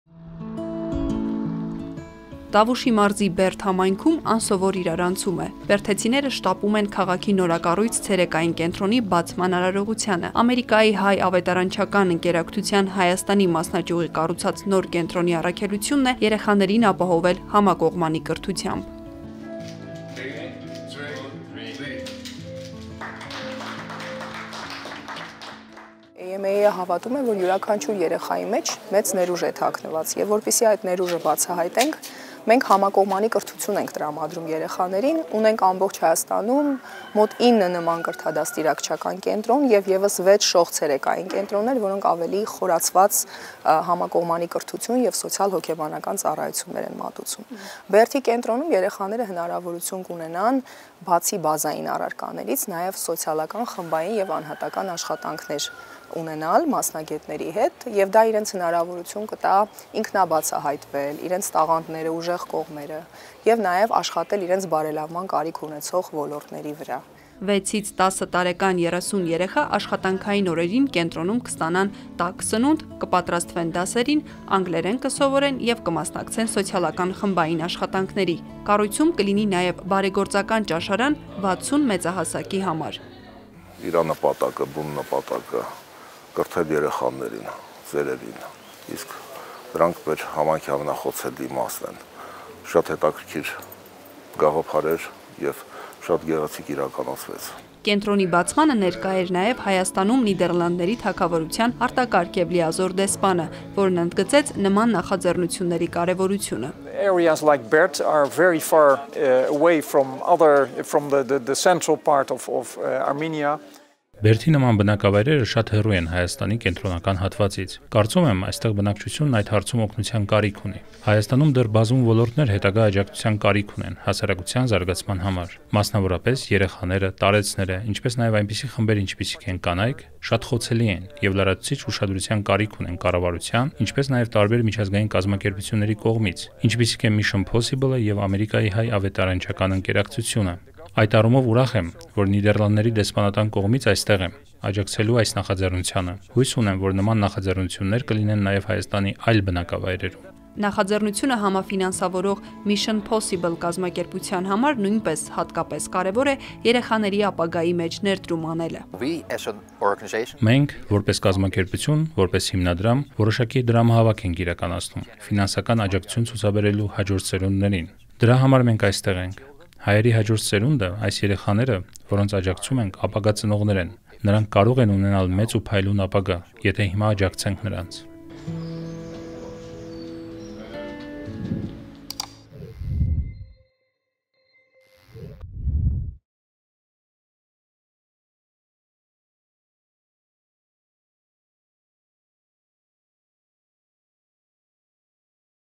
Ավետարանչականը հոգում է Բերդի տարեցների ու երեխաների մասին
Ապրիլի 14-ին Տավուշի մարզի Բերդ համայնքում տեղի ունեցավ Ամերիկայի Հայ Ավետարանչական ընկերակցության (AMAA) Հայաստանի մասնաճյուղի՝ Բերդի նորակառույց ցերեկային կենտրոնի բացման արարողությանը։ Կենտրոնի առաքելությունն է երեխաներին ապահովել համակողմանի կրթությամբ, այն է՝ գիտելիքներ, կյանքի հմտություններ, անձնային աճ և առողջ արժեքային համակարգ: